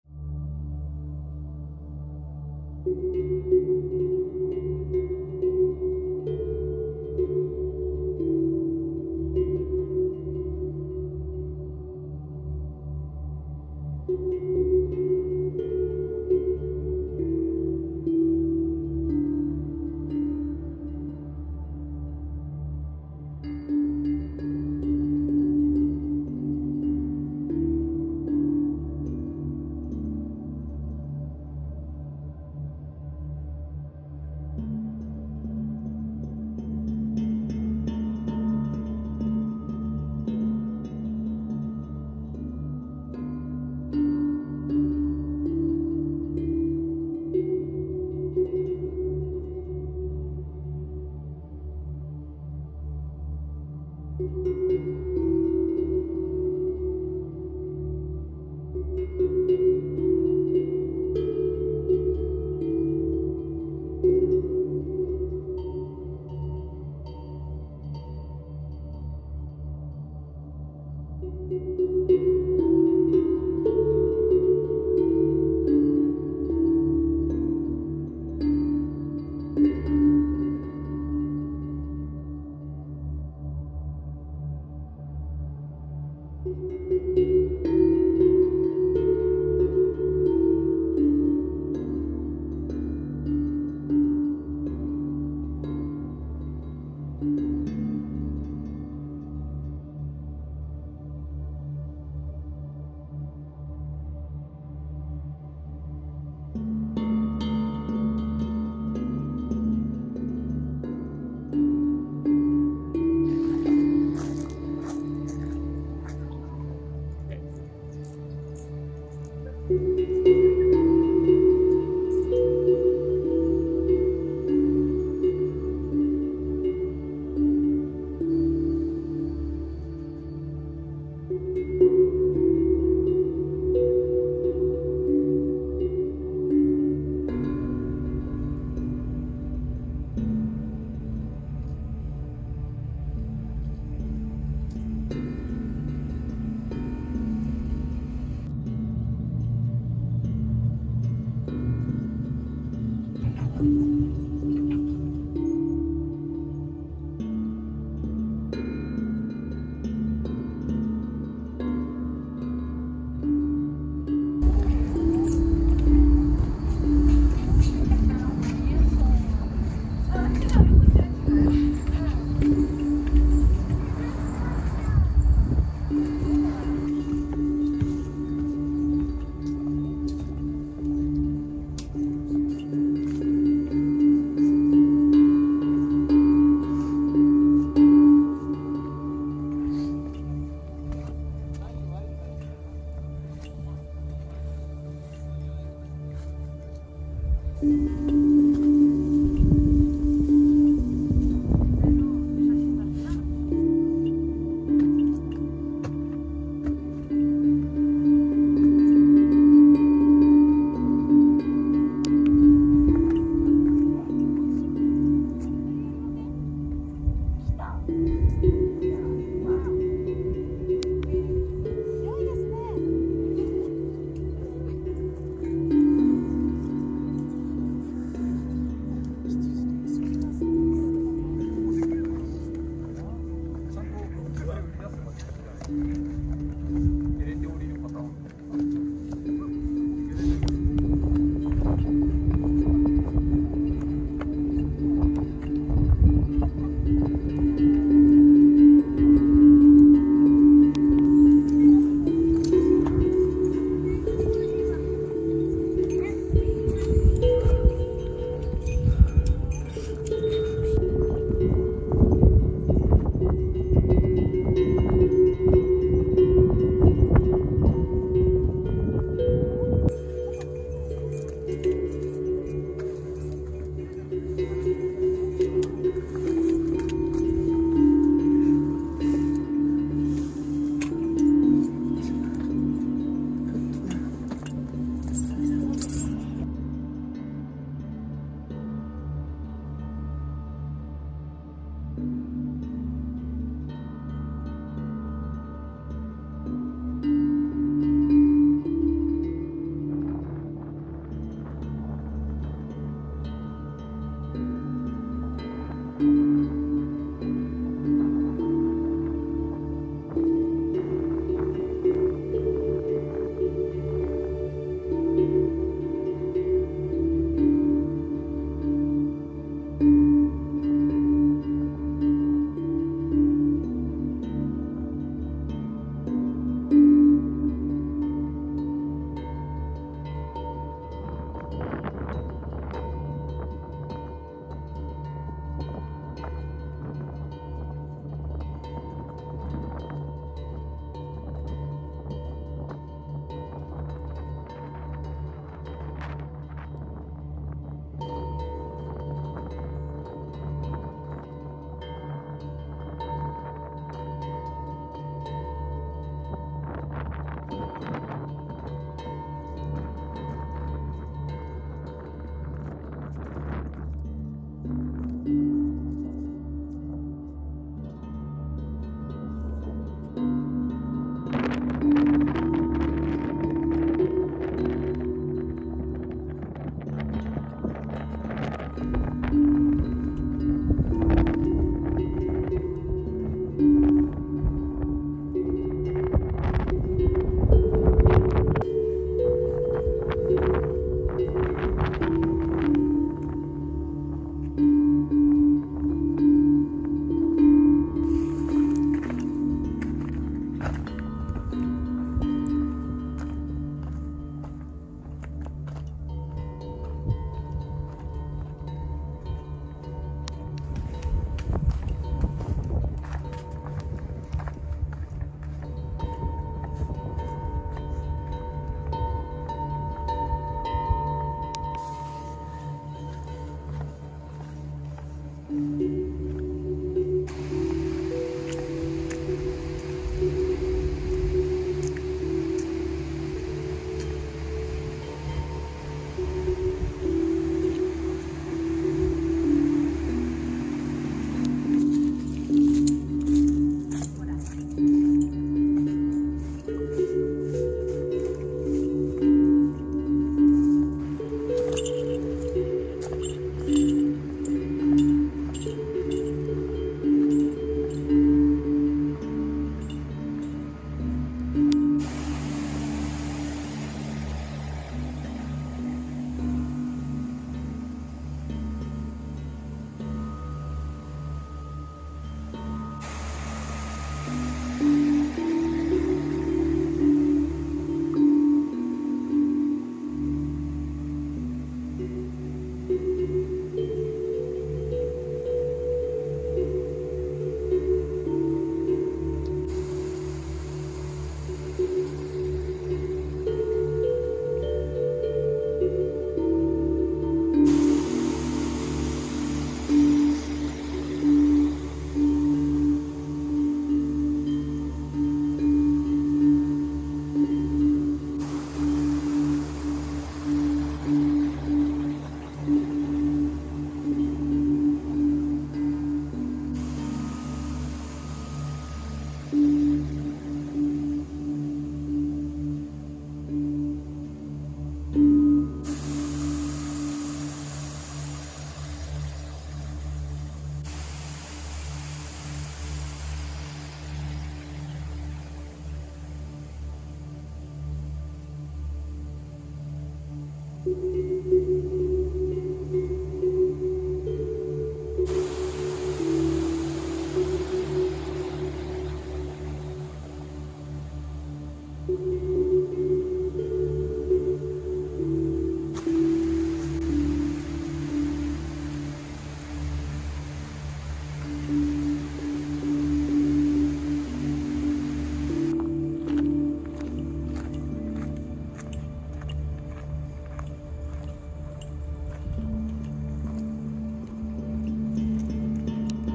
Silent day hiking Mt Adatara in Fukushima, Japan
A stratovolcano in Fukushima

Elevation: 1,728 m
Mountain range: Ōu Mountains
Last eruption: AD 1900
Shot with an Iphone 11pro

Gear: